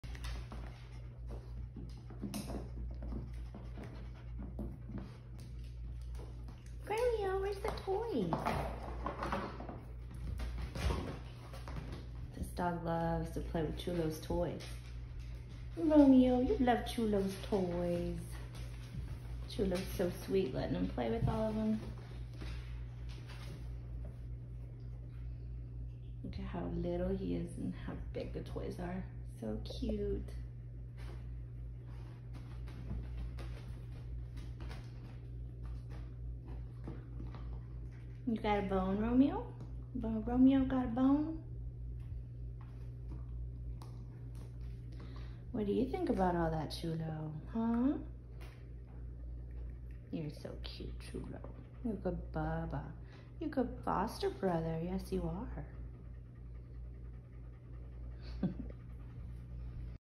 Video of adoptable pet named Romeo
Romeo is an adoptable pet with Honoring Hope and Faith Rescue, Inc. in Houston TX.